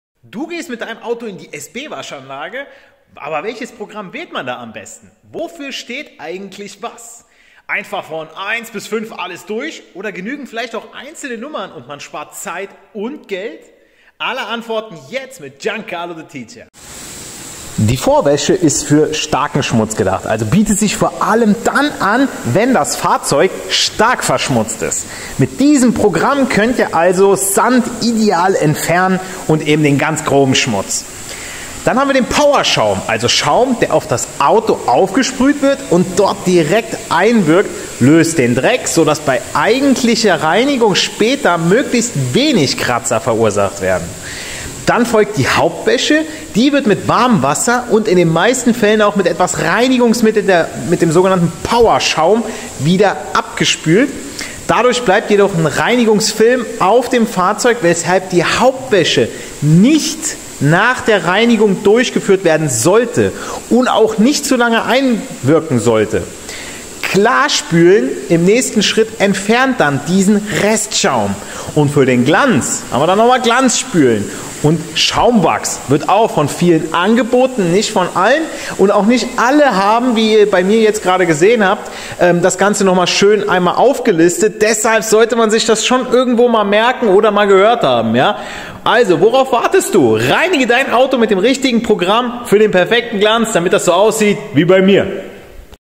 Programme der SB-Waschanlage in a Nutshell
🚗💦 Mit dem Auto in die Waschanlage: Darauf solltest Du bei den Programmen achten!

Die richtigen Schritte, um dein Auto strahlend sauber zu halten!

Hier erfährst Du alles über die verschiedenen Waschprogramme in SB-Waschanlagen. 🌟

✨ Vorwäsche: Nur bei starken Verschmutzungen notwendig!

💦 Powerschaum: Der Schmutzkiller für eine kratzfreie Reinigung!

🧽 Hauptwäsche: Warmes Wasser und Reinigungsmittel für die gründliche Reinigung!

🚿 Schaumbürste: Die Hauptreinigung mit integriertem Wasserdurchfluss!
💧 Klarspülen: Entfernt
 überschüssigen Schaum für ein glänzendes Ergebnis!

✨ Glanzspülen und Schaumwachs: Für den perfekten Glanz und eine langanhaltende Lackversiegelung!

Egal, ob Du ein Auto-Enthusiast oder ein Waschanlagen-Neuling bist, hier findest du alles, was du wissen musst, um Dein Fahrzeug strahlend sauber zu halten! 💫

Für mehr Infos zu weiteren wichtigen Inhalten rund um die Elektrotechnik, hört hier rein:

Giancarlo the Teacher
Lehrer für Elektrotechnik
Trainer für Cardio-Fitness
Ernährungstrainer